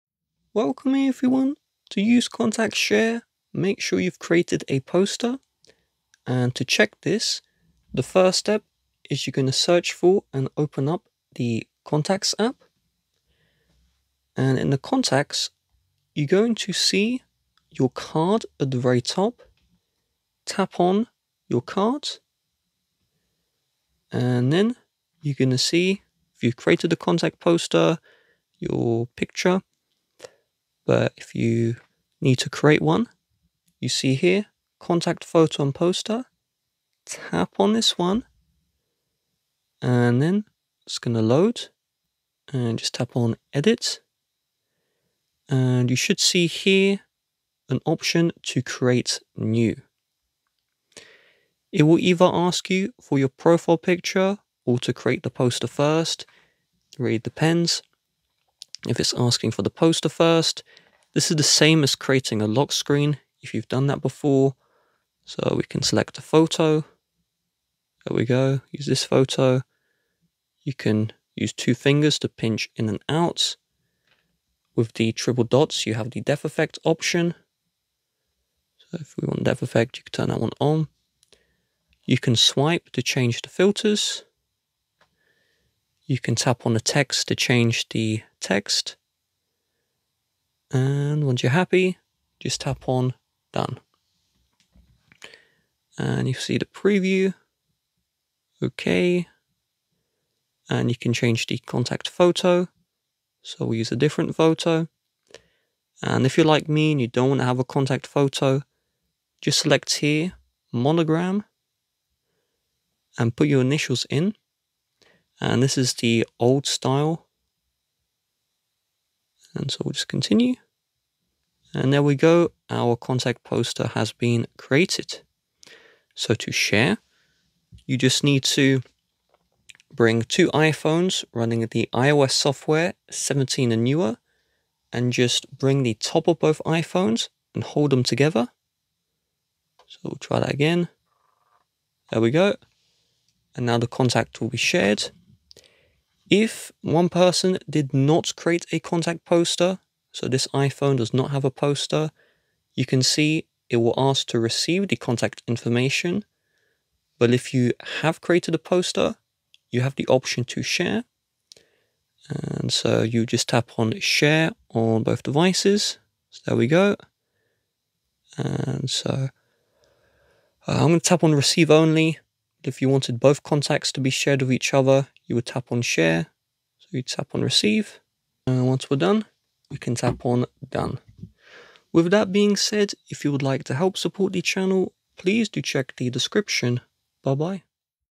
How To Use Contact Share on iPhone iOS 17
Learn how to use contact share on iPhone. Both iPhones must be running iOS 17 software update or later for the contact share to work successfully. You can't contact share between iPhone and iPad.